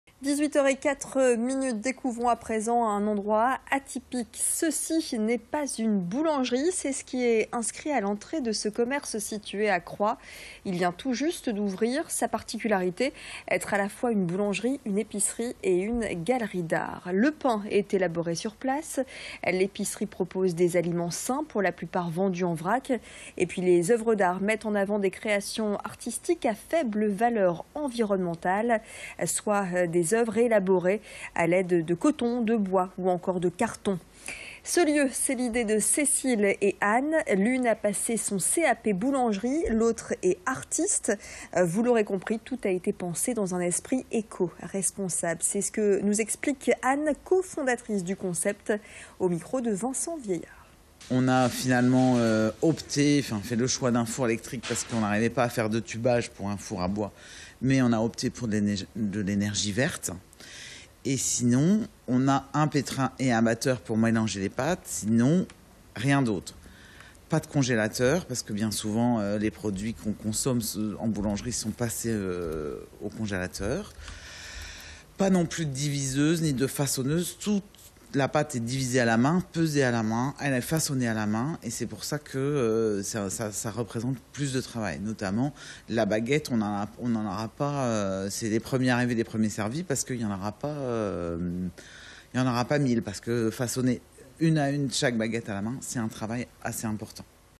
Ceci n'est pas une boulangerie
Coup de projecteur sur "Ceci n'est pas une boulangerie", un lieu qui mêle boulangerie, épicerie et galerie d'art!